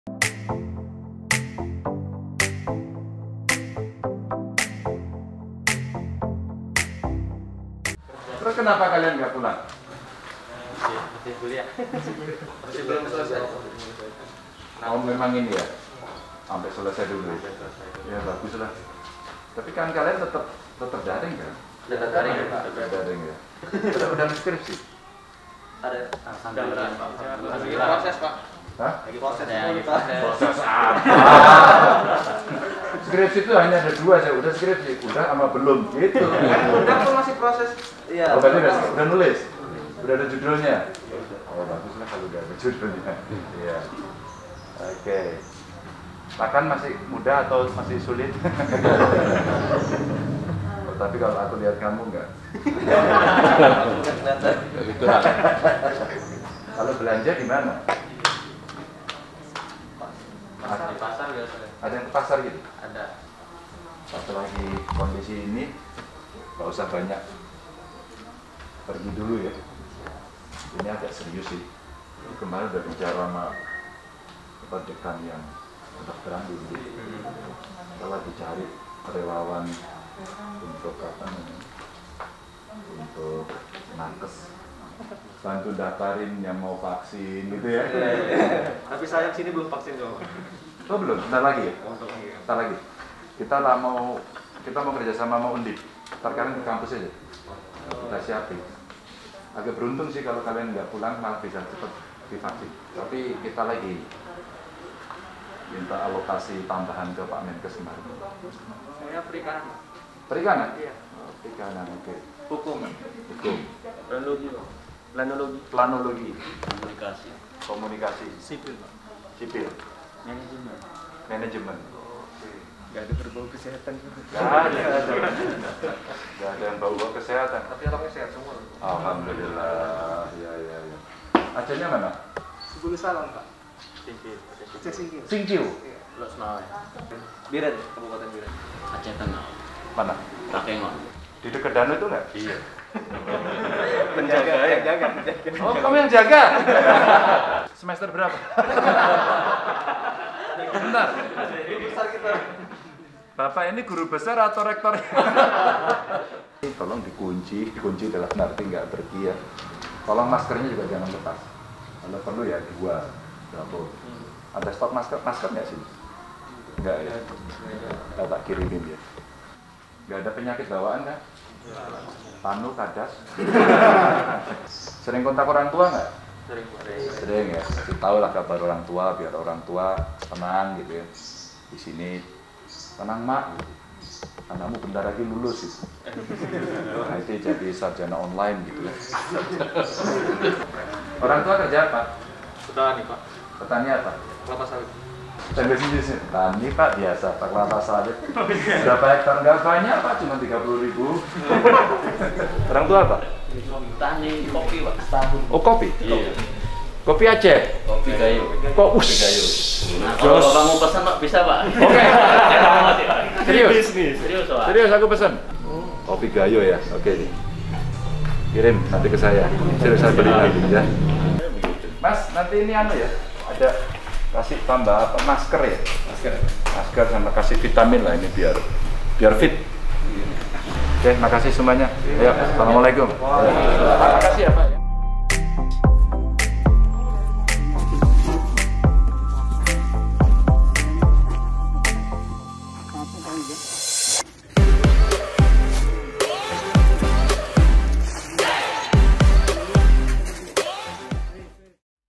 PANTANG PULANG KE ACEH SEBELUM JADI SARJANA
Sebagai mantan anak kos, saya tau bagaimana perasaan mahasiswa Aceh ini. Rasa rindu pada orang tua harus ditahan, termasuk menahan lapar ketika kiriman belum datang hehe..

Meski KTP mahasiswa ini dari Aceh, Sumatra, atau Kalimantan, selama mereka menuntut ilmu di Jawa Tengah, maka mereka jadi tanggung jawab saya.

Maka saya datangi satu persatu asrama-asrama mahasiswa luar Jawa yang ada di Semarang. Saya pastikan mereka bisa belajar dengan baik, sehat, dan yang terpenting bisa makan dengan cukup.

Saya doakan kelak setelah meraih gelar sarjana mereka bisa pulang ke Aceh dan berkontribusi membangun daerah kelahirannya.